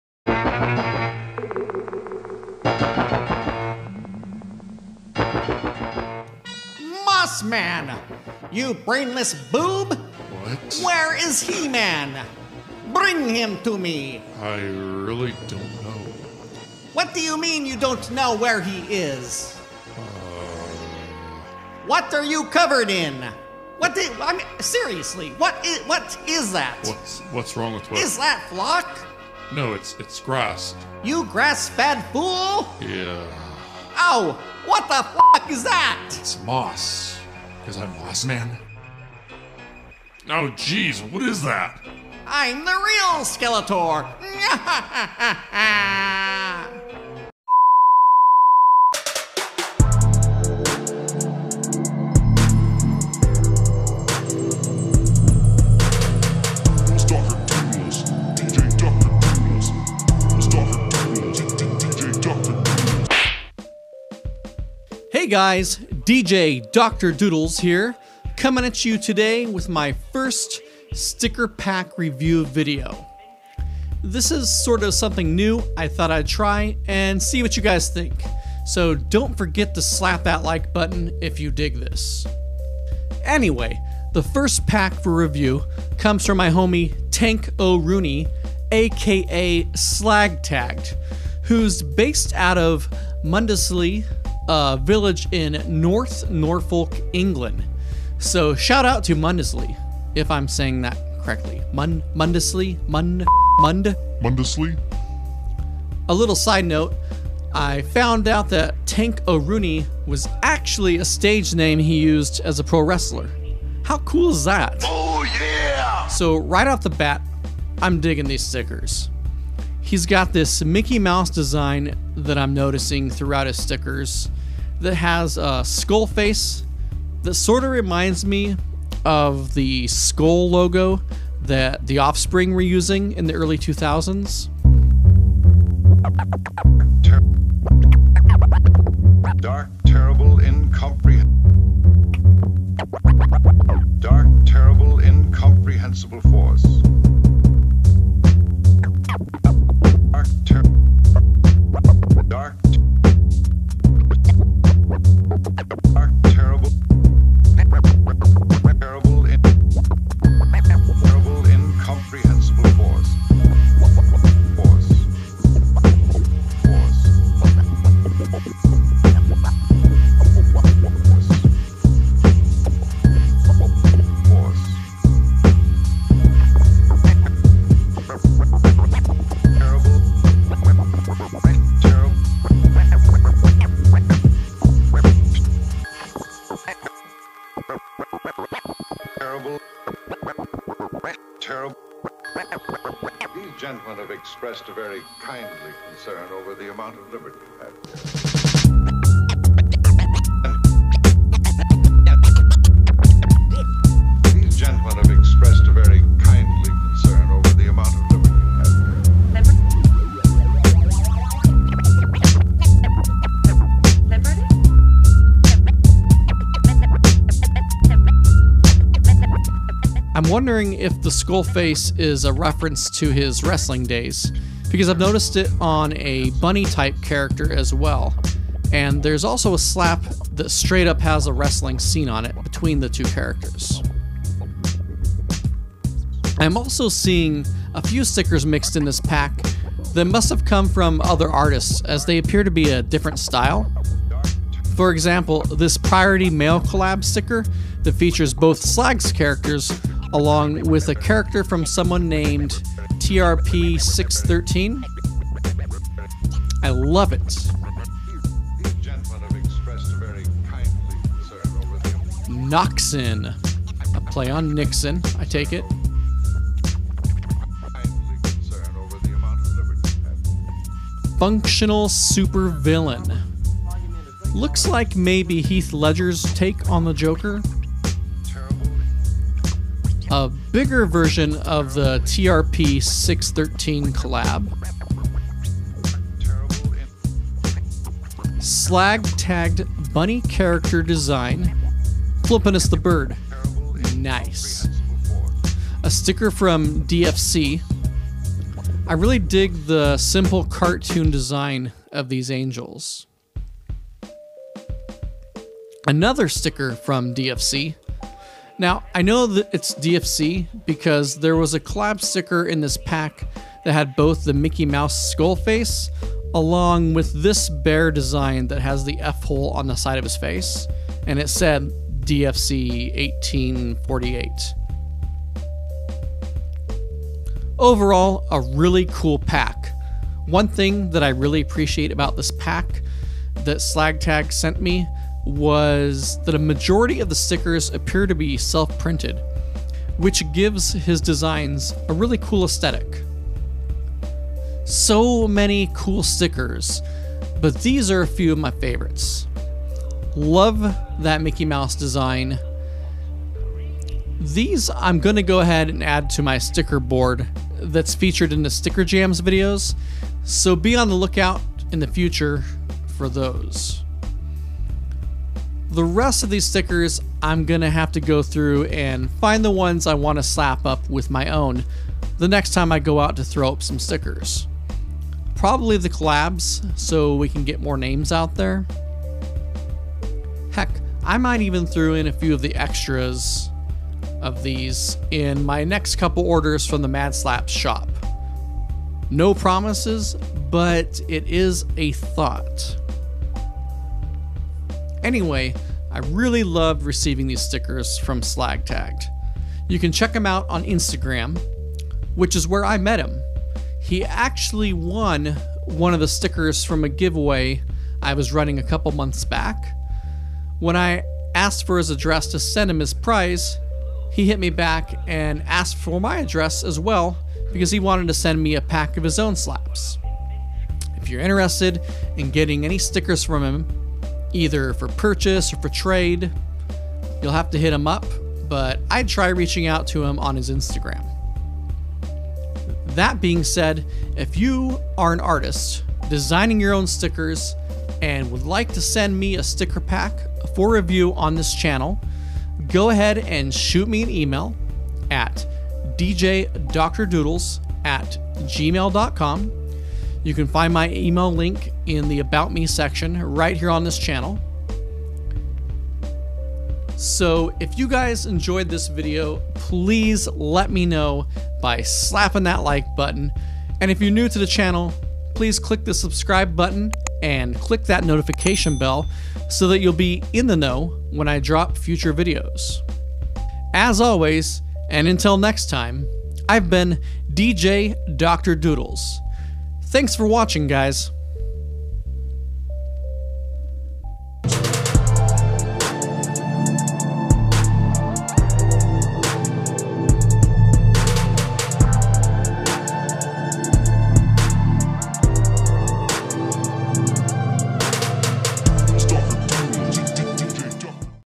Sticker Pack Review #1
My first sticker pack review video featuring the work of @slag_tagged. The main reason I made this video though was I think it's a fun way to support and contribute to the sticker community.

More Doodles!